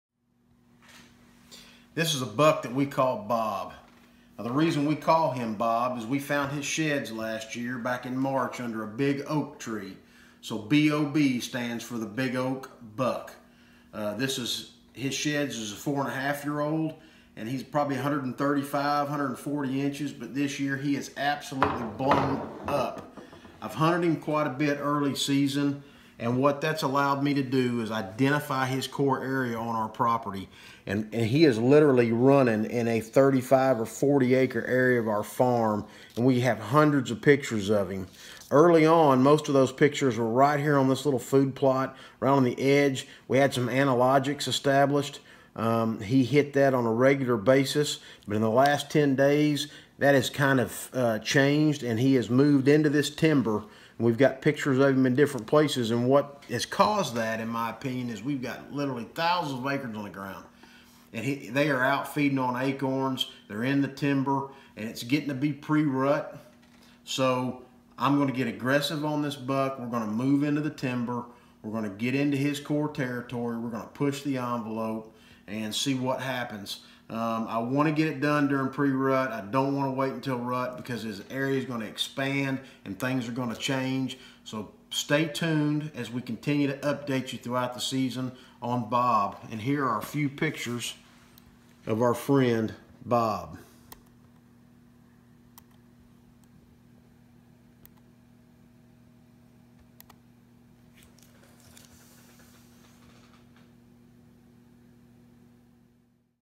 Meet Bob: Whitetail Properties Agent Mark Williams is after this buck!
Be sure to follow along as Mark Williams hunts Bob this season.

The reason we call him Bob is because we found his sheds under an oak tree last year and started call him the Big Oak Buck. I’ve hunted him quite a bit during the early season. It has allowed me to identify his core area on our farm.  He’s changed some of his eating habits in the last couple of weeks because we are getting closer to pre-rut. He’s been feasting on acorns.